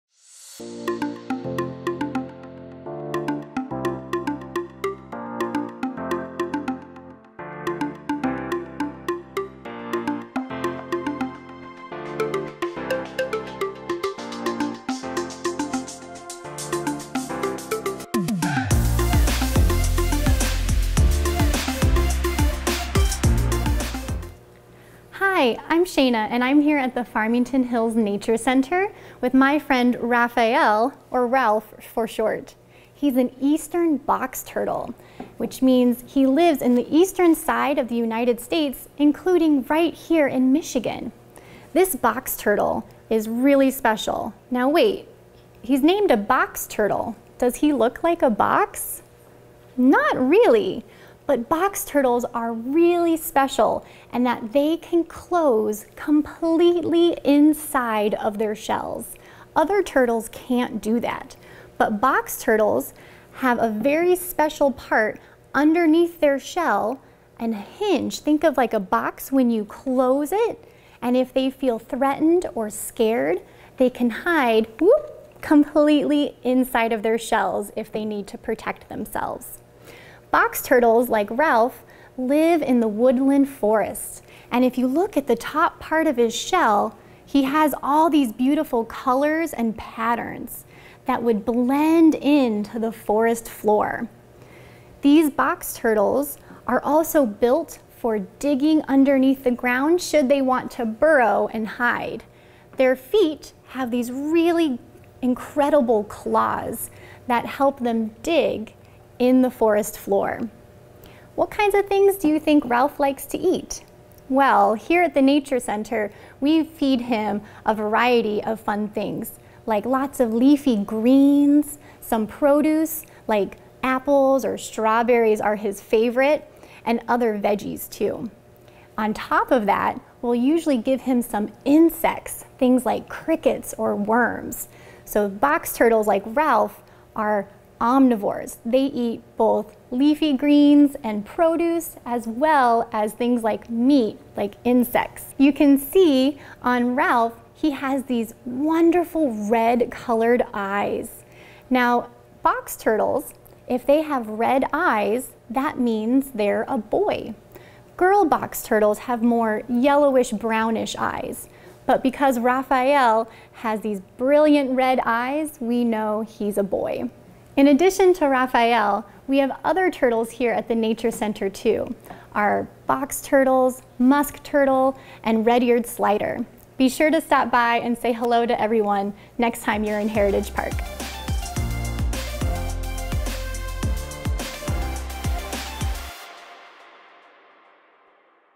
Nature Center Friends "Raphael" the Turtle
Did you know your Farmington Hills Nature Center is full of wild, furry, and fun animals? Shaina introduces us to your Farmington Hills Nature Center Friends!